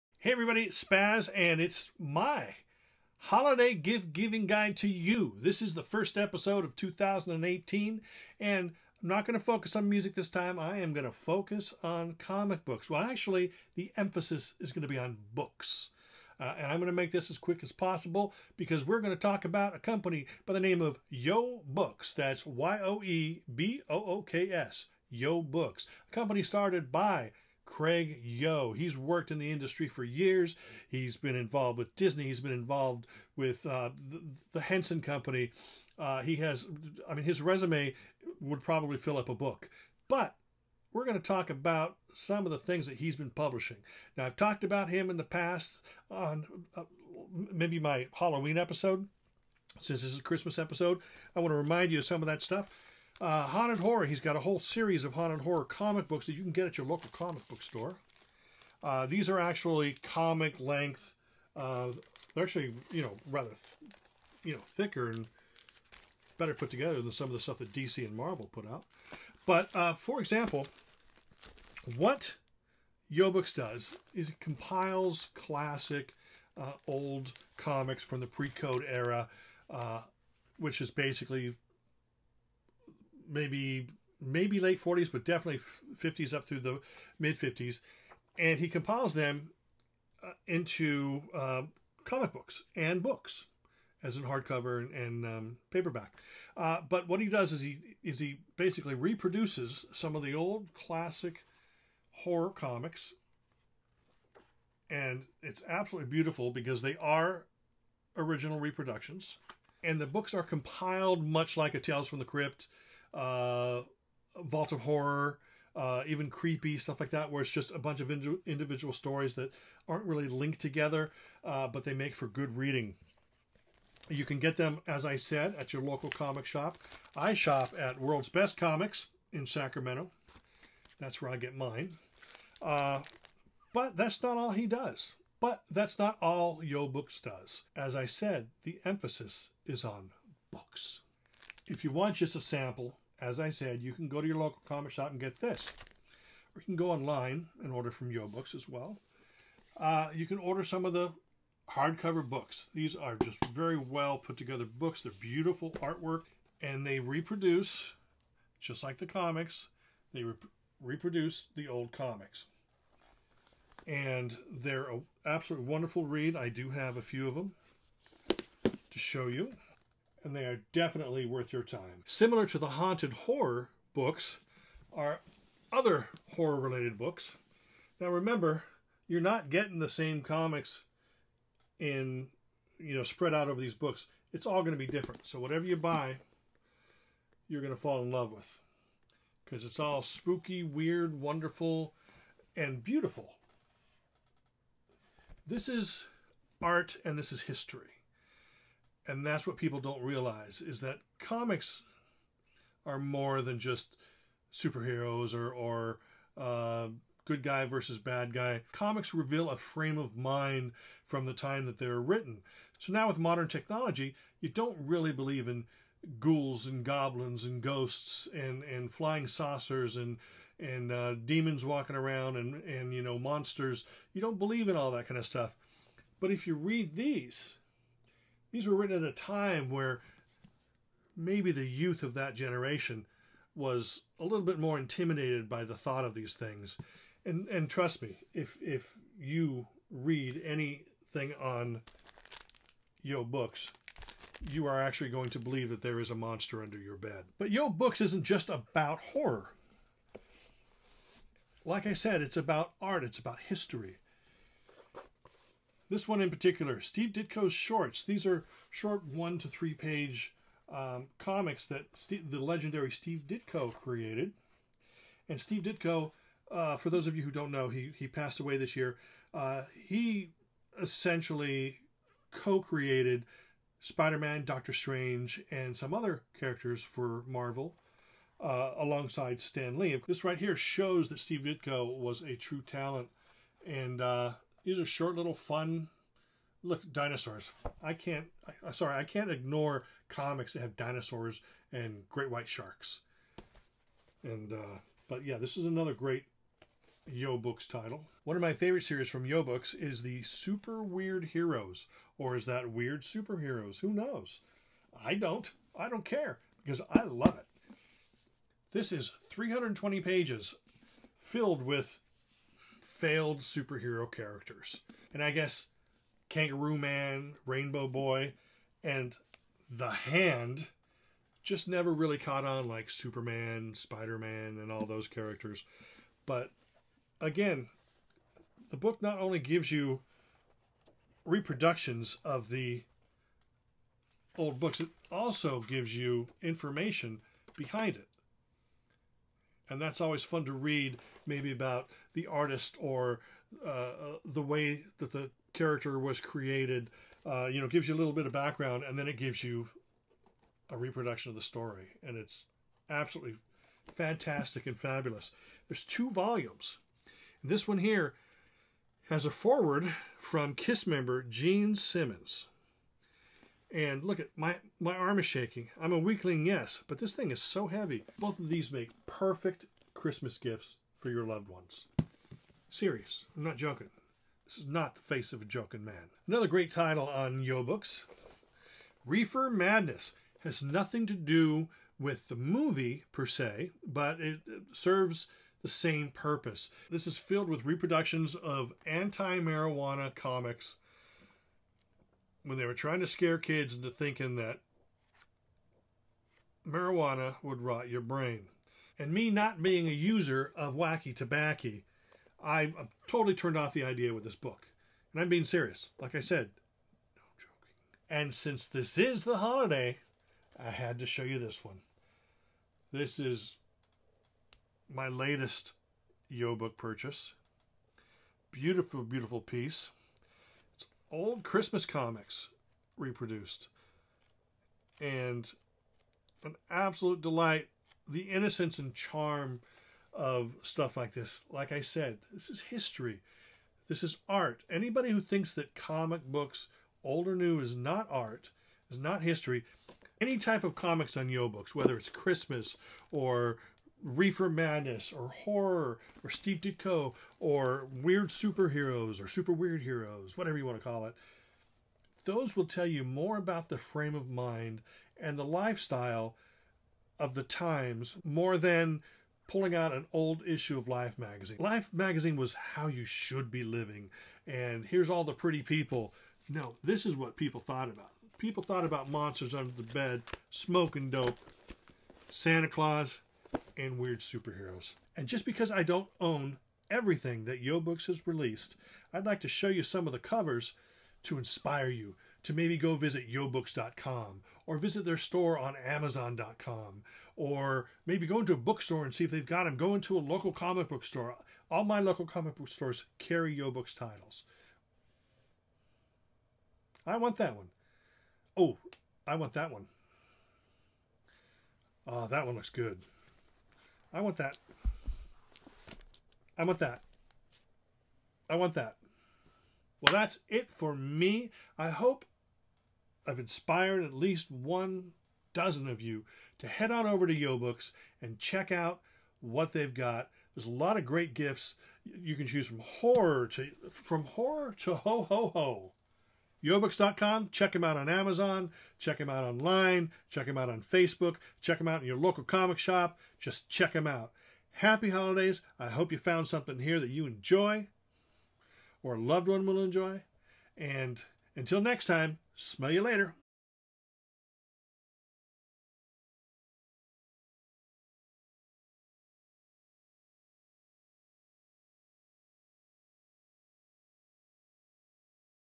SPAZ presents A Holiday Gift Giving Guide: YOE BOOKS
Spaz talks about Yoe Books and their great selection of comic-related titles!